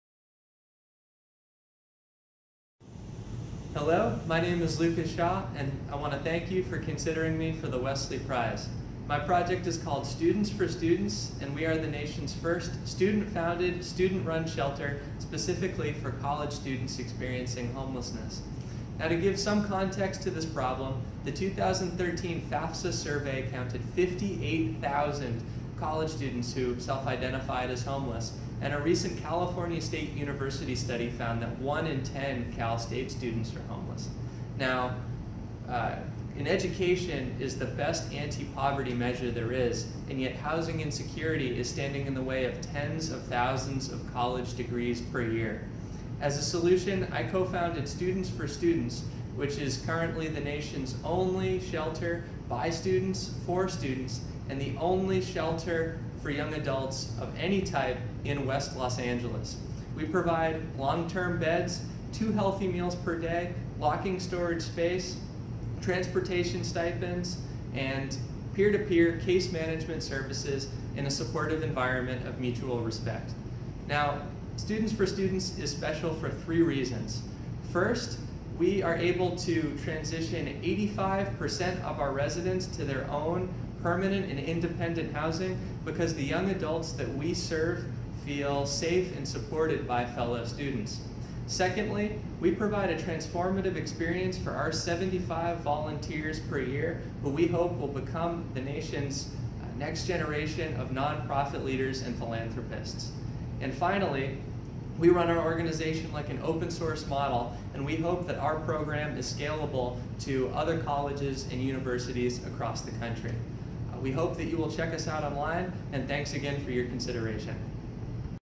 Westly Prize 2017 - Lucas Shaw of Students 4 Students
Students 4 Students, formerly known as Bruin Shelter, is the nation's first and only student-founded, student-run shelter specifically for college students experiencing homelessness. College homelessness is on the rise and standing in the way of tens of thousands of diplomas, and brighter futures, each year.

As a solution, Students 4 Students provide long-term beds, healthy meals, and peer-to-peer case management services. We are able to transition 85% of our residents to their own permanent and independent living situations. The Students 4 Students peer-to-peer model is scalable to colleges and universities worldwide.